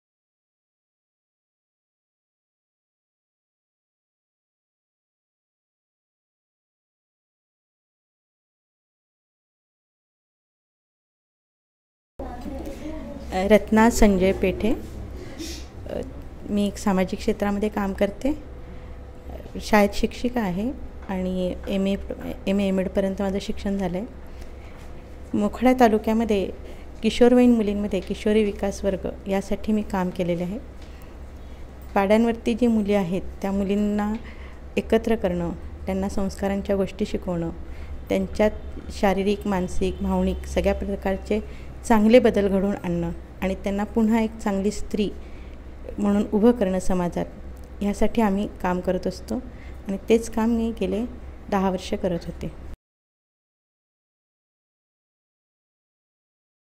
Deshdoot Karmayogini 2019 | Education | Ratna Pethe
देशदूत घेऊन येतोय कर्मयोगिनी पुरस्काराचा नजराणा. विविध क्षेत्रात कर्तृत्वाची मोहोर उमटवणाऱ्या महिलांना कृतज्ञ नमस्कार!

सन्मान स्त्री शक्तीचा... सन्मान स्त्री कर्तुत्वाचा... सन्मान स्त्री जगताचा...
पुरस्कार वितरण समारंभ २० एप्रिल २०१९ रोजी सायंकाळी ५ वाजेपासून.
ठिकाण: रावसाहेब थोरात सभागृह, गंगापूर रोड, नाशिक